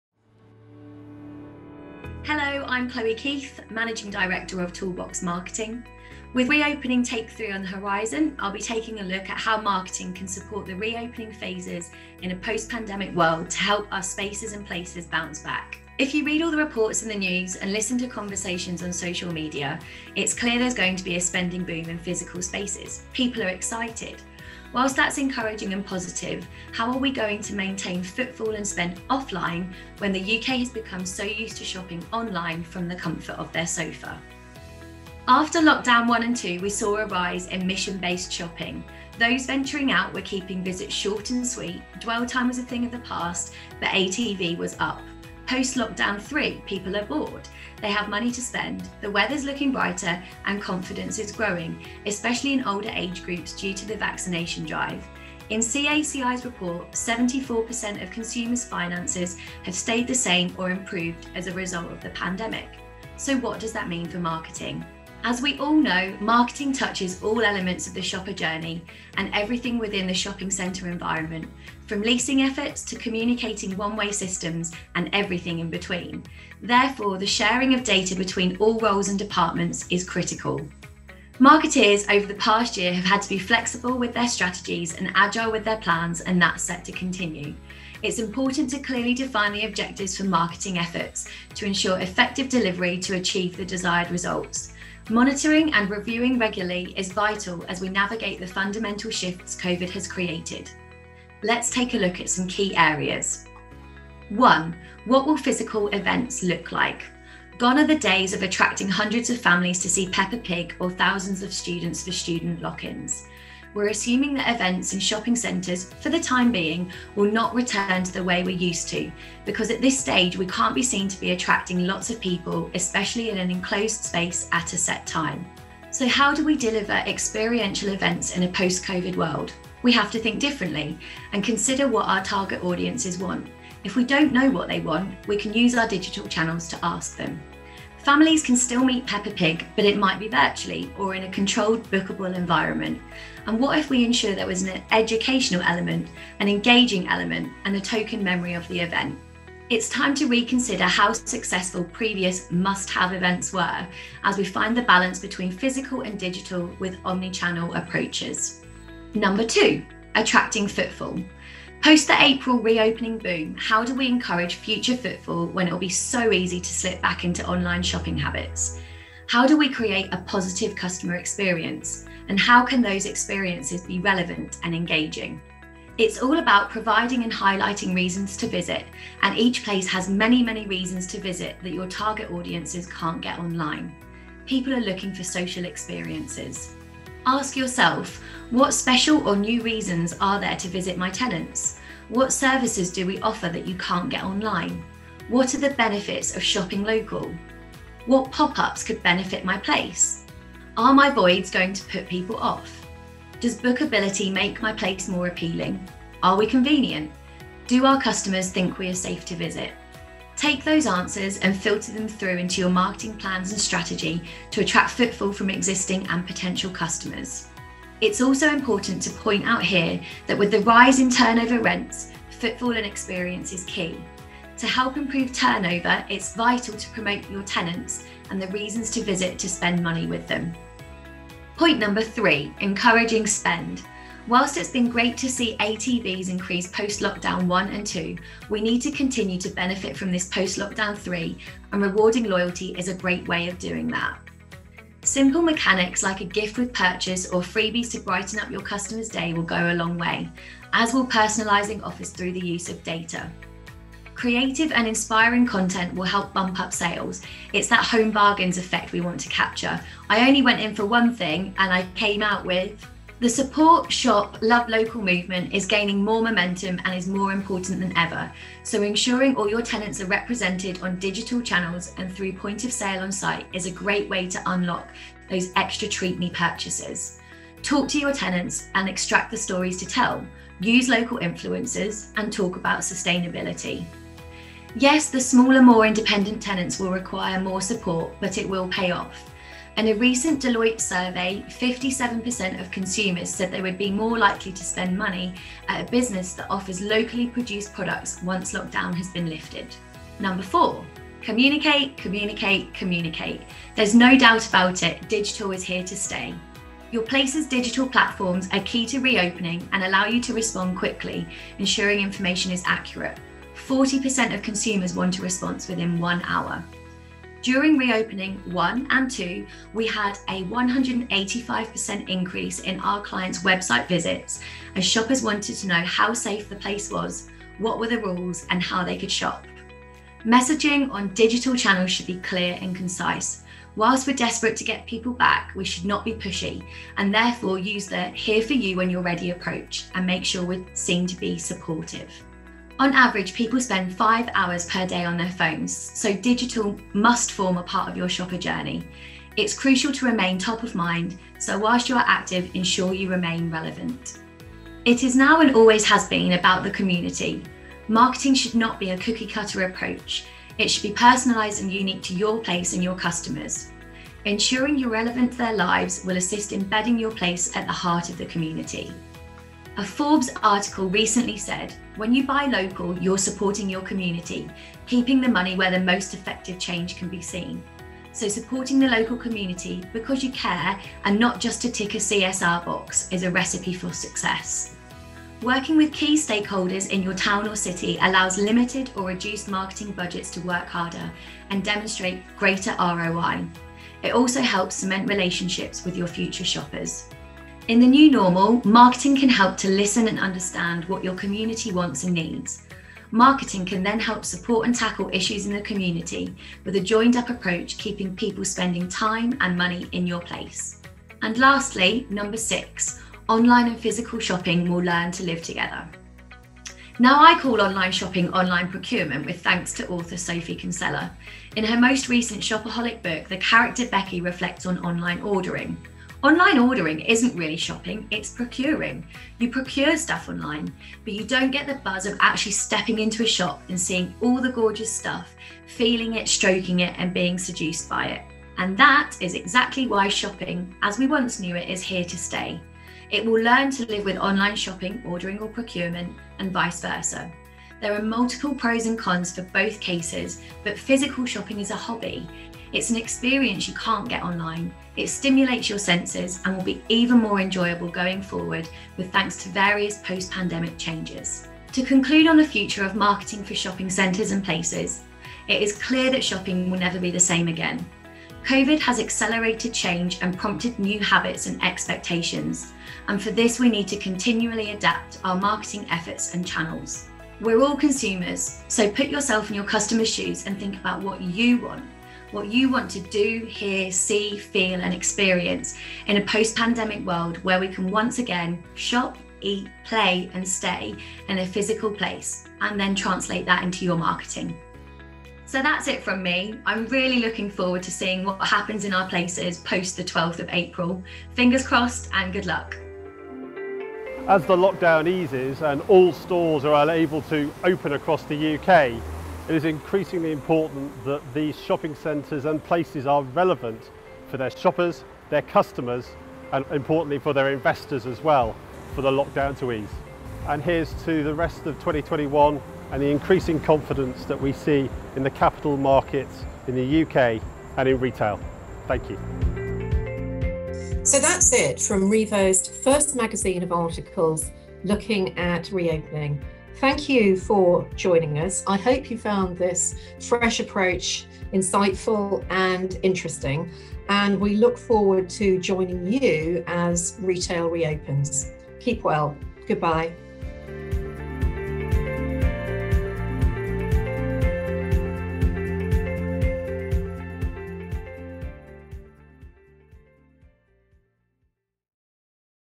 Revo Reopening Retail - Insight 6
Chloe Keith, Managing Director of Toolbox Marketing, discusses the importance of marketing during the reopening and in a post-pandemic world where things start to resemble some kind of normal for our places, tenants, customers and communities.

Thank you to all involved, including the production company, Solent Productions, and their students at Solent University.